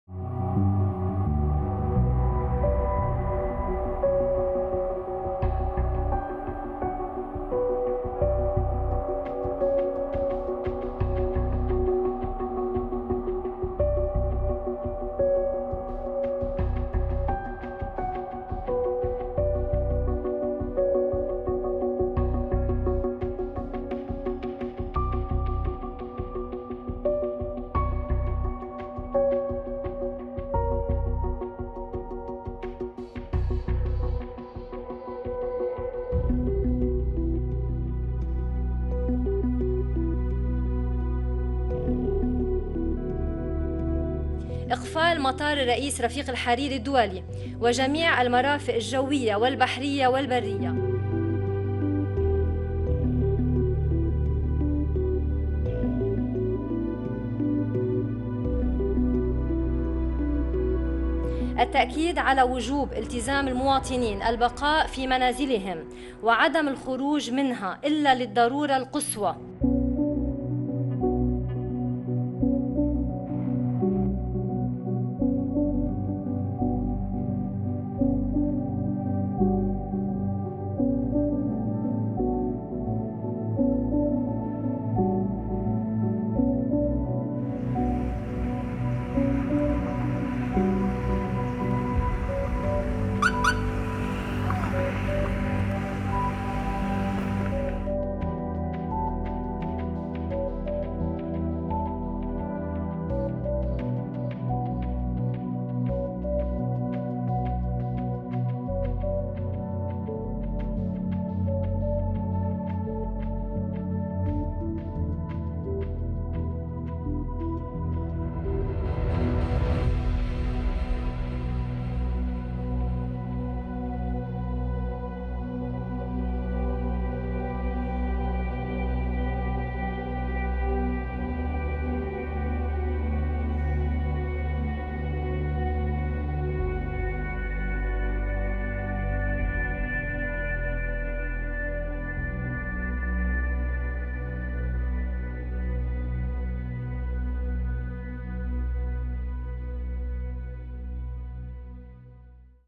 شوارع لبنان شبه خالية من المارة والسيارات .. والسبب فيروس كورونا
رصدت مشاهد جوية كيف بدت بيروت مع إعلان حظر التجول الذاتي، فمع إعلان رئيس الحكومة حسان دياب ارتفاع عدد الاصابات بفيروس كورونا في لبنان.

شرعت السلطات في تنفيذ خطة عسكرية أمنية لنشر القوى على الأرض في كل المناطق اللبنانية وإطلاق دوريات وإقامة حواجز بما يوازي حال طوارئ غير معلنة ومنع التجول واتخاذ الإجراءات كافة لإلزام المواطنين بالتزام عدم مغادرة منازلهم، بدت بيروت صباح اليوم، شبه خالية من السكان مع انتشار العناصر الأمنية على الطرق، فيما خلت الشوارع من السيارات الا فيما ندر.